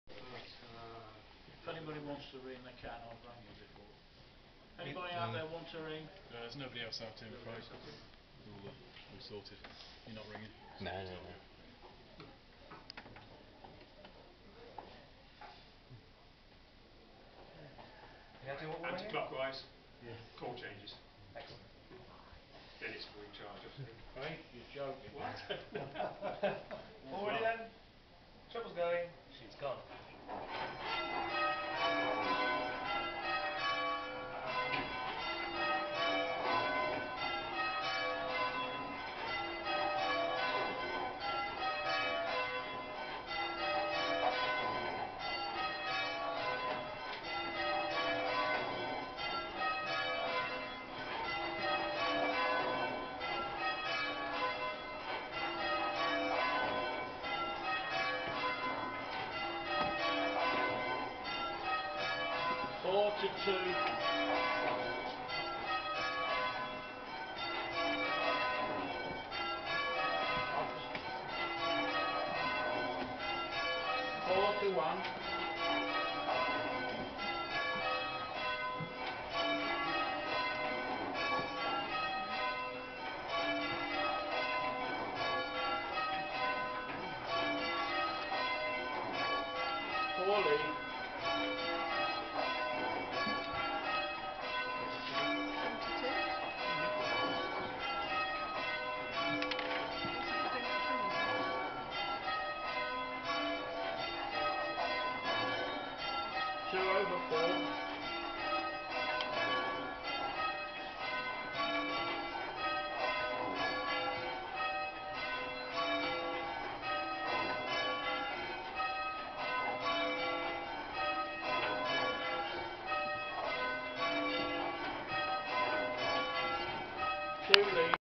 Ringing at St Bartholomew, Smithfield.
Ringing the Oldest complete ring of bells in the country.